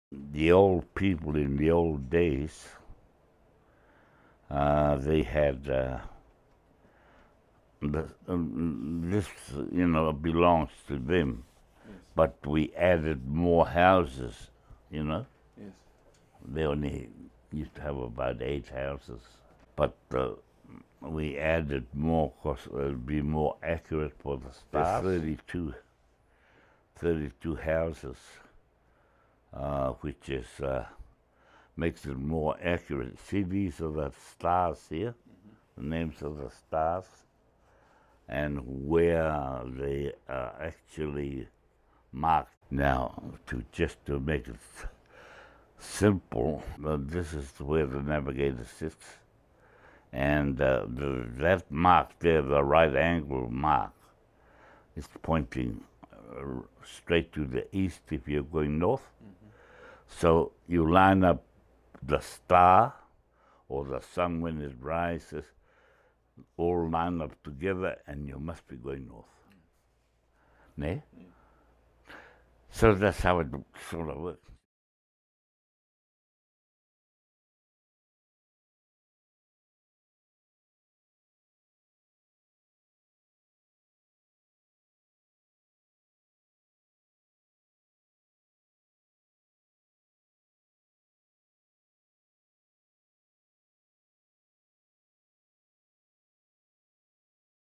Tā Hec Busby’s Te kāpehu whetū, the Māori star compass
Early this year, the Education Gazette has had the privilege to interview world-renowned navigator and master waka builder Tā Hekenukumai Puhipi Busby. In this video, Tā Hec talks about his star compass and how this navigation tool helps the navigator keep the waka on course by observing the location of the sun, moon and stars throughout the journey.

Read more on Education Gazette: Voyaging from the past into the future